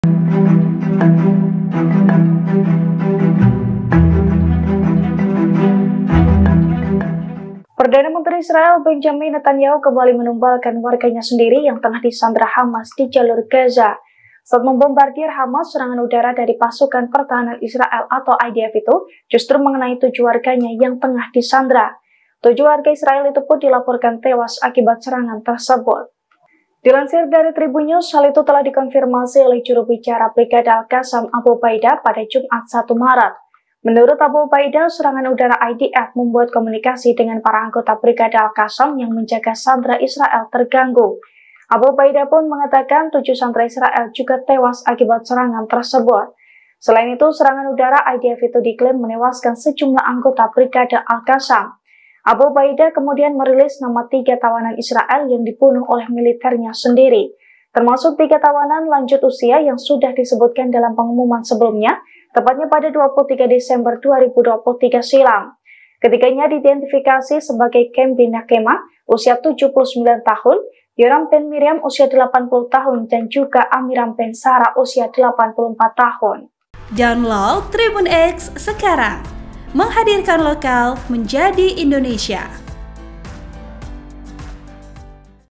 🔴 Serangan Udara Gaza Blunder Lagi Netanyahu Disalahkan, Sandera Israel Ikut Tewas
TRIBUN-TIMUR.COM - Serangan Udara Gaza Blunder Lagi Netanyahu Disalahkan, Sandera Israel Ikut Tewas

(TRIBUN-TIMUR.COM)